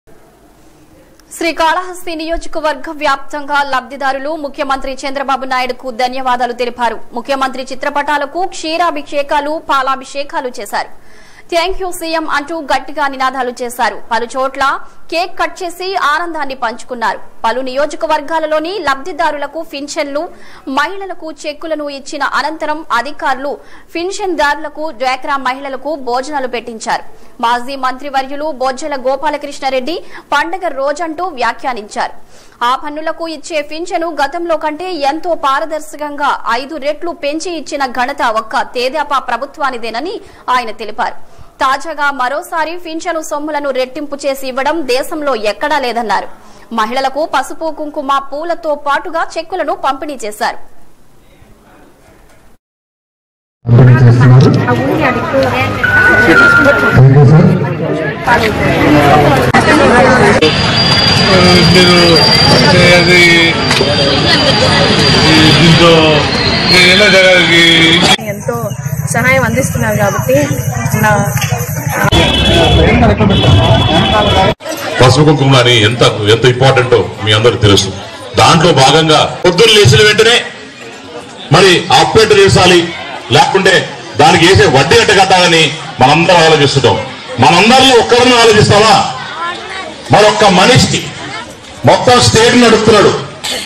శ్రీకాళహస్తీనియోజవర్గంవ్యాప్తంగా లబ్ధిదారులు ముఖ్యమంత్రి చంద్రబాబునాయుడుకు
శ్రీకాళహస్తీనియోజవర్గంవ్యాప్తంగా లబ్ధిదారులు ముఖ్యమంత్రి చంద్రబాబునాయుడుకు ధన్యవాదాలు తెలిపారు. ముఖ్యమంత్రి చిత్రపటాలకు క్షీరాభిషేకాలు పాలాభిషేకాలు చేశారు. థ్యాంక్యూ సీఎం అంటూ గట్టిగా నినాదాలు చేశారు. పలు చోట్ల కేక్‌ కట్‌చేసి ఆనందాన్ని పంచుకున్నారు. పలు నియోజకవర్గాల్లో లబ్ధిదారులకు పింఛన్లు, మహిళలకు చెక్కులు ఇచ్చిన అనంతరం అధికార్లు పింఛన్లు, దారులకు డ్వాక్రామహిళలకు భోజనాలు పెట్టించారు. మాజీమంత్రి వర్యులు బోజ్జల గోపాలక్రిష్ణ రేడ్డి  పండగ రోజంటూ వ్యాఖ్యానించారు. ఆపన్నులకు ఇచ్చే పింఛను గతంలో కంటే ఎంతో పారదర్శకంగా.. ఐదు రెట్లు పెంచి ఇచ్చిన ఘనత ఒక్క తెదేపా ప్రభుత్వానిదేనని.. తాజాగా మరోసారి పింఛను సొమ్ములను రెట్టింపు చేసి ఇవ్వడం దేశంలో ఎక్కడా లేదన్నారు. మహిళలకు పసుపు-కుంకుమ పూలతో పాటుగా చెక్కులను పంపిణీ చేశారు.  .